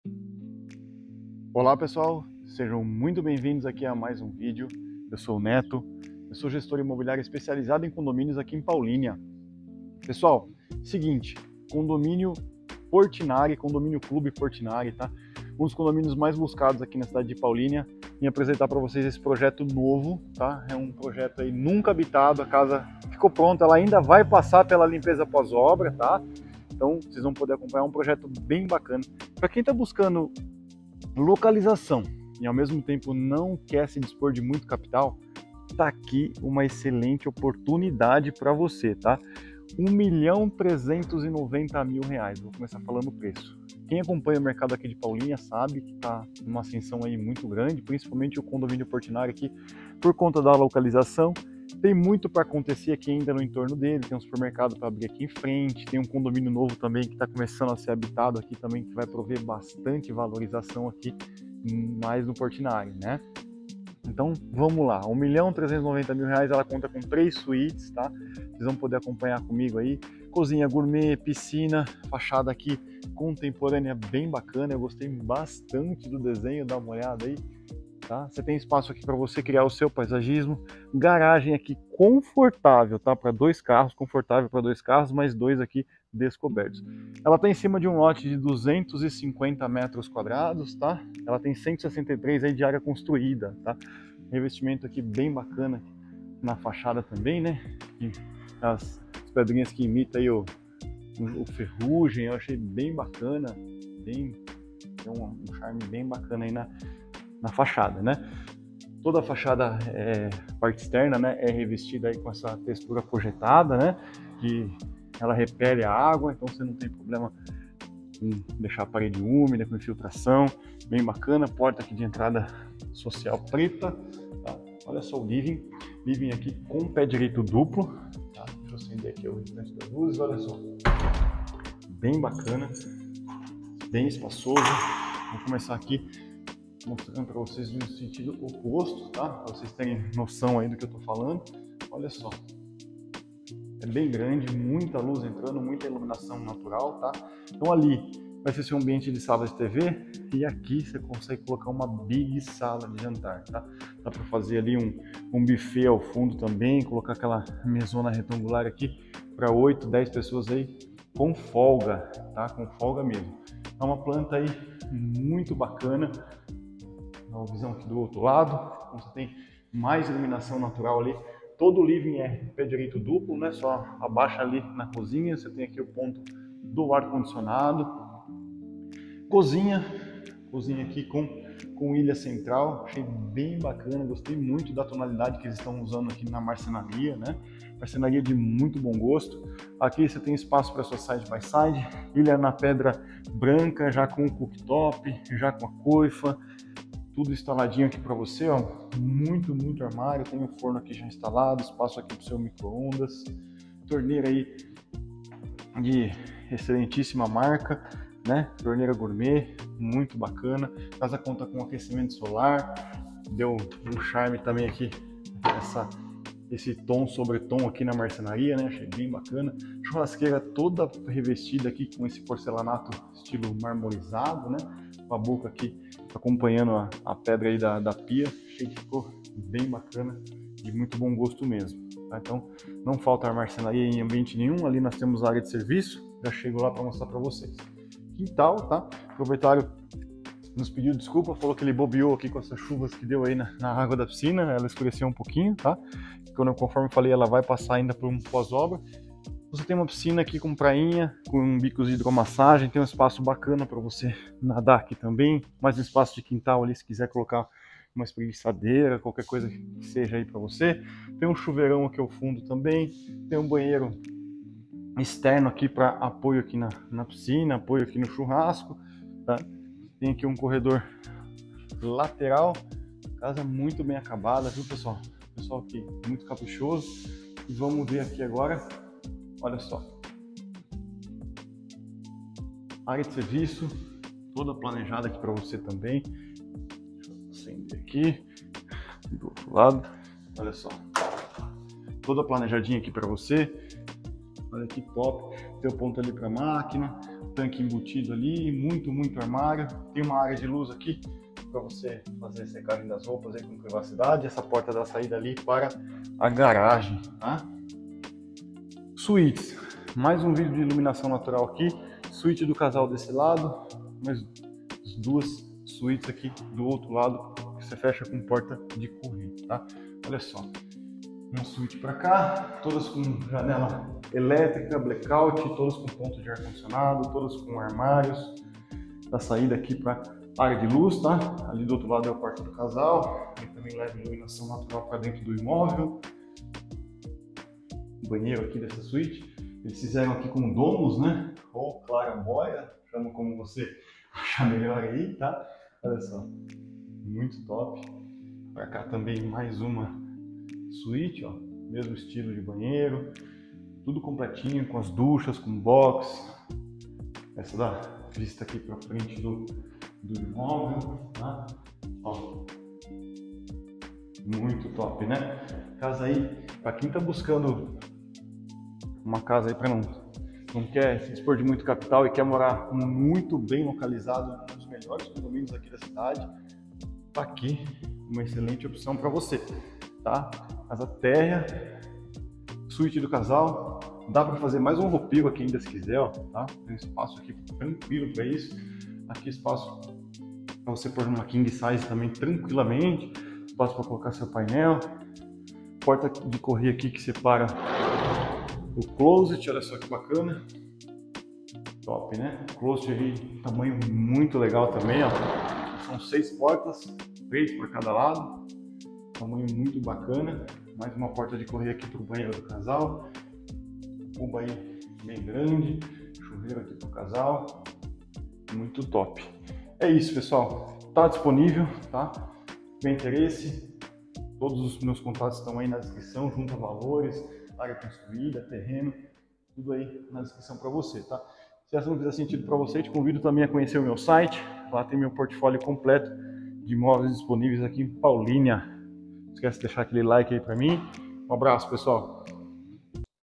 CASA TÉRREA À VENDA NO COND. MAIS DESEJADO COM 03 SUÍTES | COZINHA GOURMET
CASA TÉRREA COM 03 SUÍTES | À VENDA EM PAULÍNIA- 03 suítes com armários-Sala de TV -Sala de Jantar-Lavabo -Cozinha Gourmet com Ilha central e rica em armários- Área de serviço com armários-Piscina-Garagem para 04 carros *Alguns diferenciais: toda a marmoraria da casa foi feita com Quartzo Branco, esquadrias automatizadas, piscina de pastilha com hidro, todos os metais e louças são Deca, pisos 93x93, planta integrada e com bastante iluminação natural, arquitetura moderna- Ao lado das principais rodovias, fica apenas a 15 minutos do Shopping Dom Pedro - 📐163,00m2 Construção - 📐250,00m2 Terreno- Condomínio - R$ 650,00/ mêsAGENDE JÁ SUA VISITA COMIGO E VENHA CONFERIR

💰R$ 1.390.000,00

Portfólio completo

Código : CA01231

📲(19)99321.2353

Corretor Neto
CRECI: 218859-F